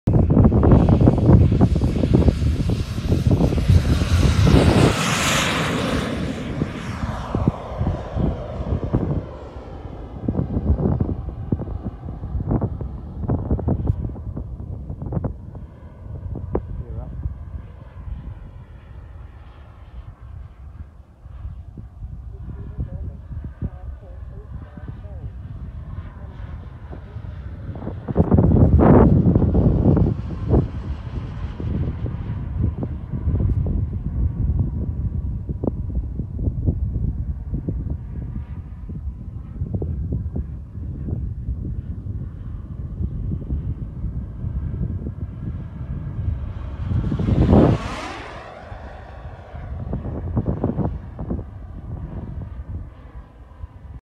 My T-one Fortune
She stonks along with a Jetcat 160 Easter Sunday at Bicester Model Flying Club.